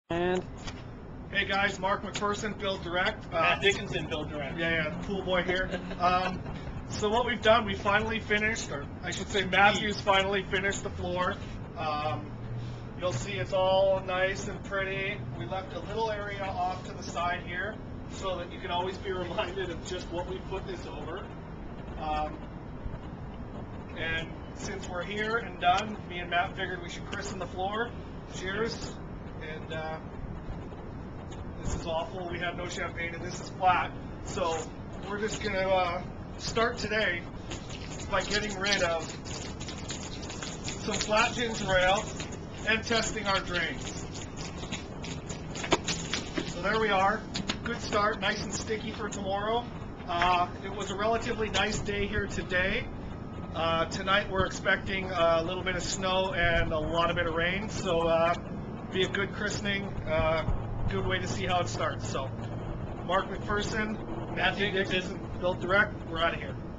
Outdoor Laminate Flooring Installation - The Christening!
Our outdoor laminate flooring is in - and since we didn't have any Champagne, we used almost-flat ginger ale.